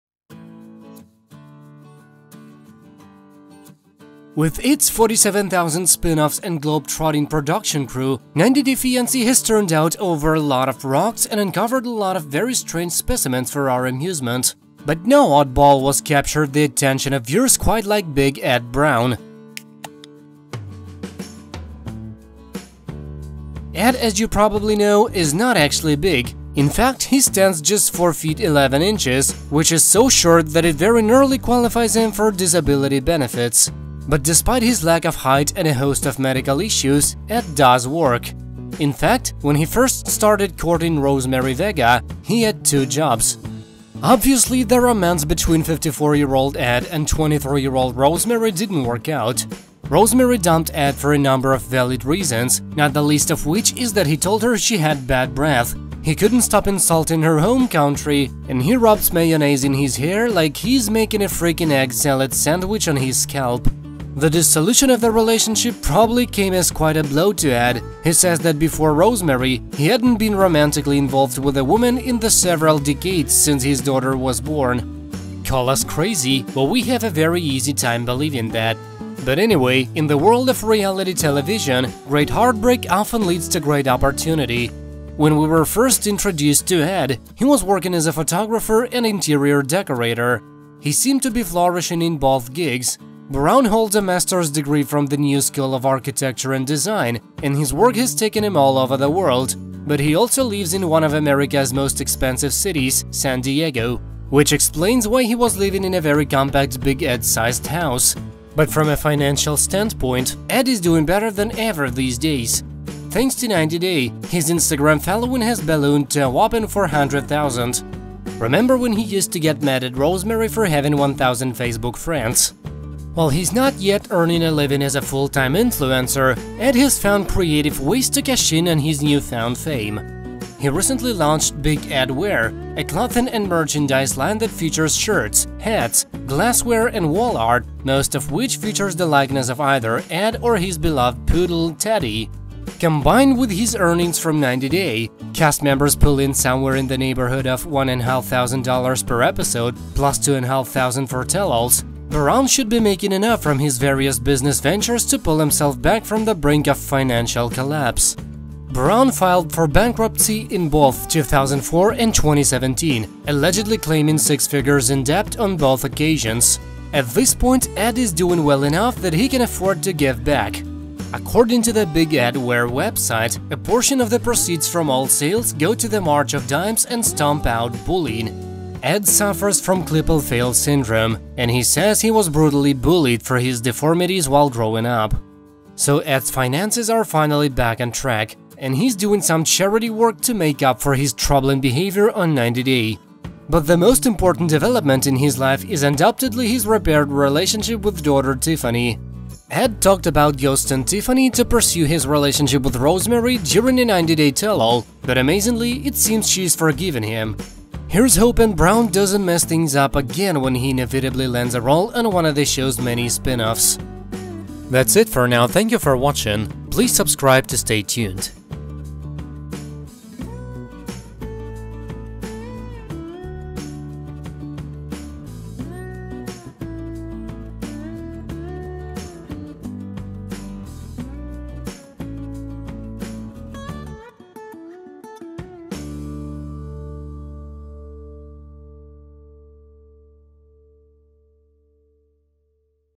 What Does Ed Brown Do For a Living? 90 day fiance before the 90 days big ed
With its 47,000 spinoffs and globe-trotting production crew, 90 Day Fiance has turned out over a lot of rocks and uncovered a lot of very strange specimens for our amusement.

But no oddball was captured the attention of viewers quite like Big Ed Brown.

Big Ed Brown at the Tell All
Ed, as you probably know, is not actually big.

In fact he stands just 4'11", which is so short that it very nearly qualifies him for disability benefits.

But despite his lack of height and a host of medical issues, Ed does work.

In fact, when he first started courting Rosemarie Vega, he had two jobs.

Big Ed Brown Speaks on B90 Strikes Back
Obviously, the romance between 54-year-old Ed and 23-year-old Rosemarie didn't work out.

(Rosemarie dumped Ed for a number of valid reasons, not the least of which is that he told her she had bad breath, he couldn't stop insulting her home country, and he rubs mayonnaise in his hair like he's making a freakin' egg salad sandwich on his scalp.)

The dissolution of the relationship probably came as quite a blow to Ed, who says that before Rosemarie, he hadn't been romantically involved with a woman in the several decades since his daughter was born.

Big Ed Brown Faces the Truth
Call us crazy, but we have a very easy time believing that.

But anyway, in the world of reality television, great heartbreak often leads to great opportunity.

When we were first introduced to Ed, he was working as a photographer and an interior decorator.

He seemed to be flourishing in both gigs -- Brown holds a master's degree from the New School of Architecture and Design, and his work has taken him all over the world -- but he also lives in one of America's most expensive cities, San Diego.

Big Ed Brown Waits
Which explains why he was living in a very compact, Big Ed-sized house.

But from a financial standpoint, Ed is doing better than ever these days.

Thanks to 90 Day, his Instagram following has ballooned to a whopping 400,000.

(Remember when he used to get mad at Rosemarie for having 1,000 Facebook friends?)

Big Ed Brown
While he's not yet earning a living as a full-time influencer, Ed has found creative ways to cash in on his newfound fame.

He recently launched BigED Wear, a clothing and merchandise line that features shirts, hats, glassware and wall art, most of which features the likeness of either Ed or his beloved poodle, Teddy.

Combined with his earnings from 90 Day, (cast members pull in somewhere in the neighborhood of $1,500 per episode, plus $2,500 for Tell Alls), Brown should be making enough from his various business ventures to pull himself back from the brink of financial collapse.

Big Ed Brown Tries to Speak 90 day fiancé
(Brown filed for bankruptcy in both 2004 and 2017, allegedly claiming six-figures in debt on both occasions.)

At this point Ed is doing well enough that he can afford to give back.

According to the BigEd Wear website, a portion of the proceeds from all sales go to the March of Dimes and STOMP Out Bullying.

Ed suffers from Klippel Feil syndrome, and he says he was brutally bullied for his deformities while growing up.

Big Ed Brown Speaks
So Ed's finances are finally back on track, and he's doing some charity work to make up for his troubling behavior on 90 Day. 90 day fiance before the 90 days

But the most important development in his life is undoubtedly his repaired relationship with daughter Tiffany.

Ed talked about ghosting Tiffany to pursue his relationship with Rosemarie during a 90 Day Tell All, but amazingly, it seems she's forgiven him.

Here's hoping Brown doesn't mess things up again when he inevitably lands a role on one of the show's many spin-offs.  reality tv